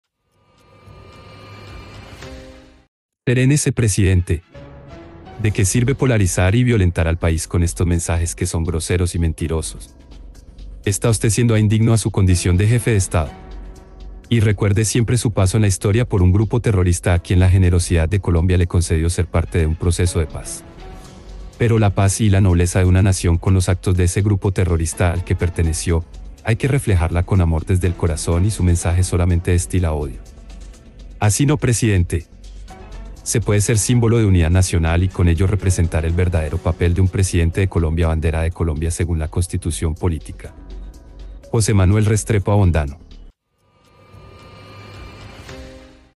¡ASÍ NO PRESIDENTE! 🔴 FUERTE MENSAJE JOSÉ MANUEL RESTREPO ABONDANO
Serénese Presidente !!! De que sirve polarizar y violentar al país con estos mensajes que son groseros y mentirosos. Está usted siendo a indigno a su condición de Jefe de Estado.